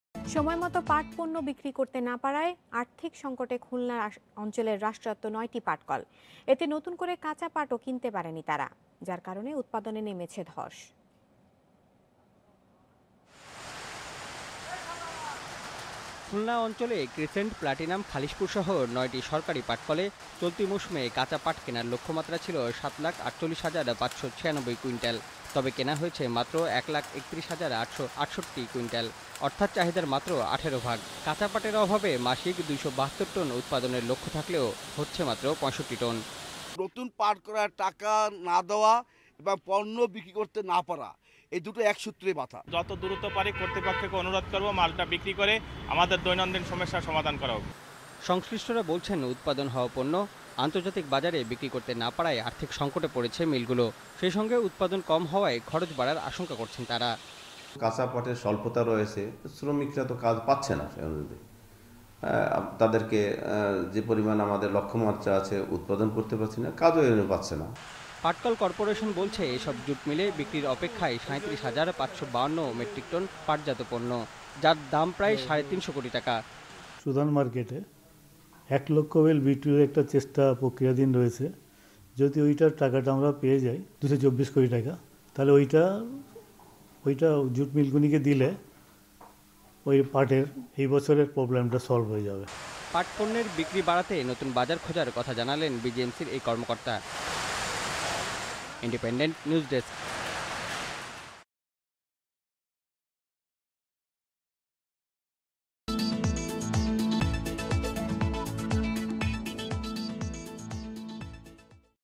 9 state-owned jute mills in financial shortage (21-01-2019) Courtesy: Independent TV
9 state-owned jute mills in financial shortage (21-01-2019) Courtesy: Independent TV